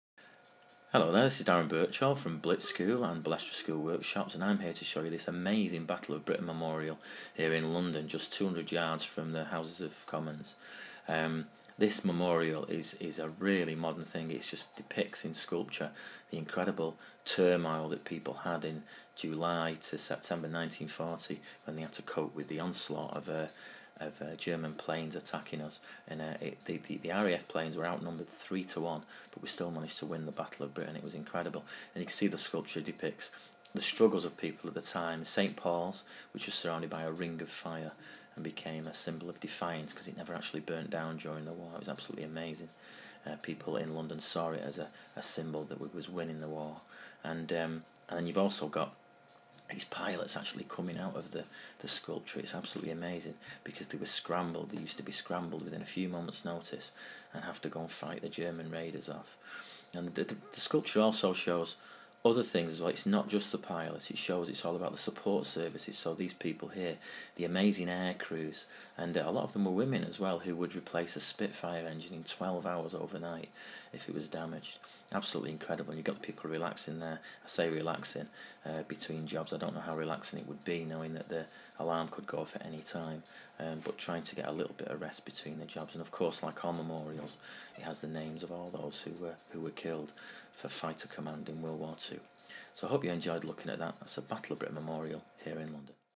Battle of Britain memorial courtesy of Blitz school workshops
A brief look around the Battle of Britain memorial in Westminster in London, brought to you provigind fun WW2 school workshops to UK primary schools.